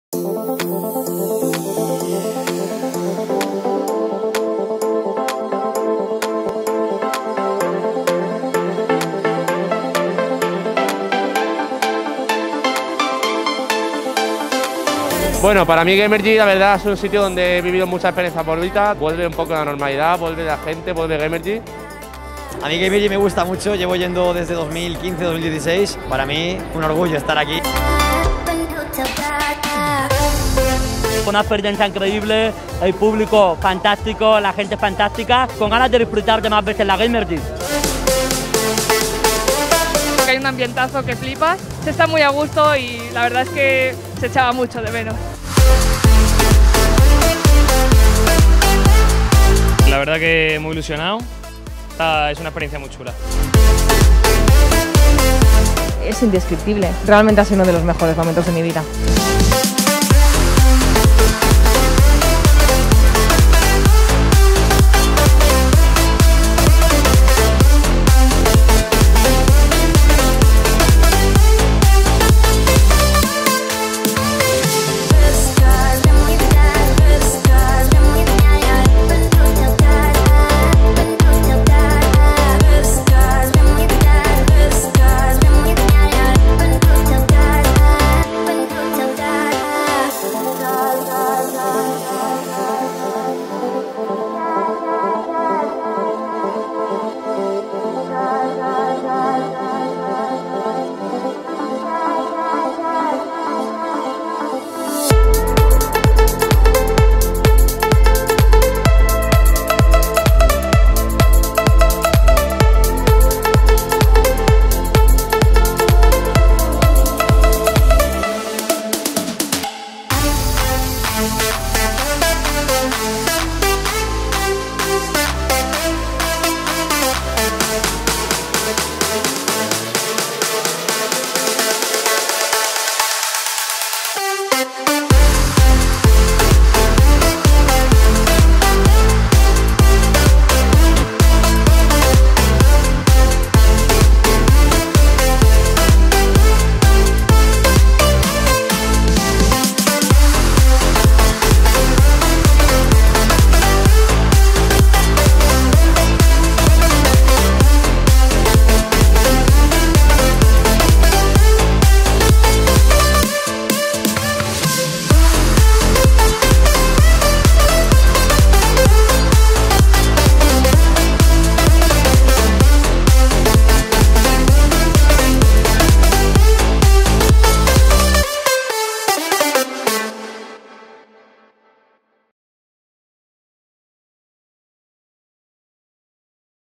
Aftermovie (versión larga)
¡Aftermovie de Amazon GAMERGY MAPFRE Edition,  celebrado los días 17, 18 y 19 de diciembre presencialmente en IFEMA MADRID!

Amazon Gamergy Mapfre Edition es un encuentro co-organizado por IFEMA MADRID y GGTech Entertainment.